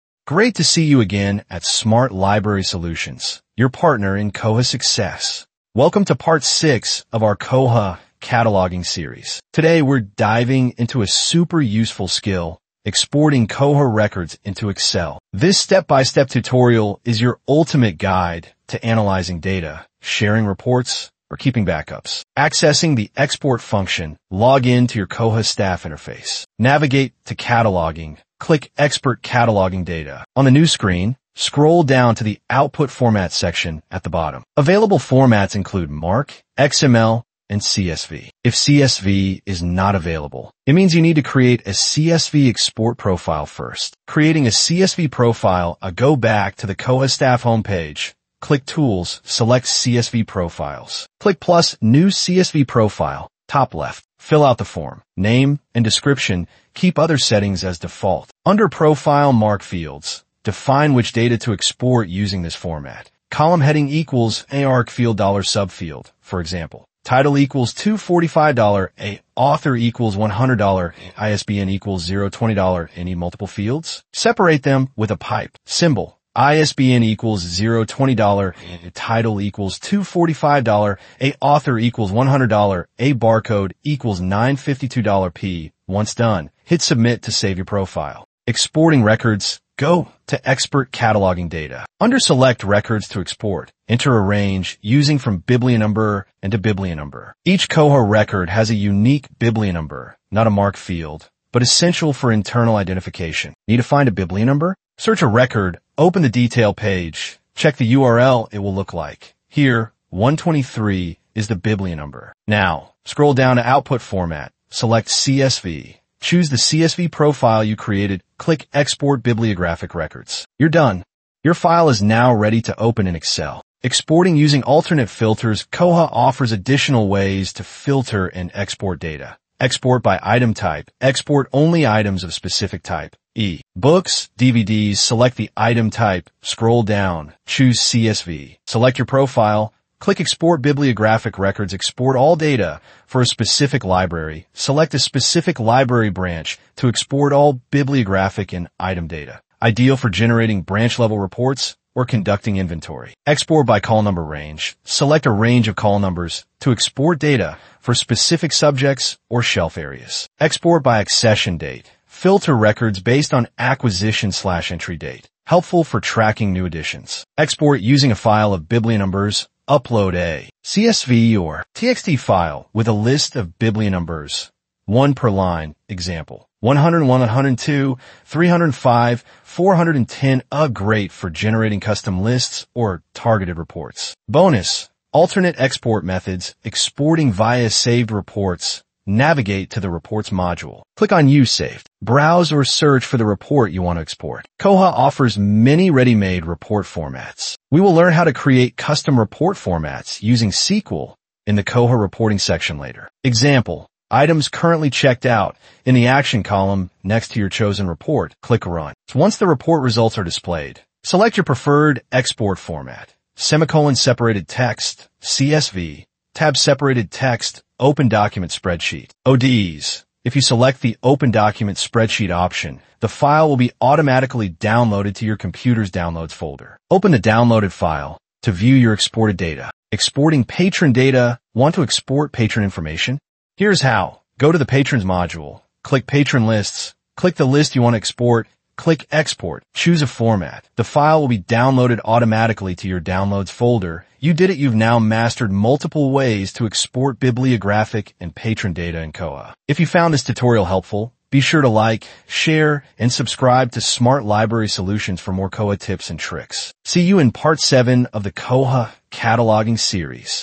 How to Export Koha Records into Excel | Koha Cataloguing Series | Part 6
In this tutorial, learn step-by-step how to export bibliographic or patron records from the Koha Library Management System into Excel (CSV format).

📌 What you'll learn:
00:00 Inro
00:22 Accessing the Export Function
00:43 Creating a CSV Profile
01:28 Exporting Records
02:03 Exporting Using Alternate Filters
03:04 Exporting via Saved Reports
03:49 Exporting Patron Data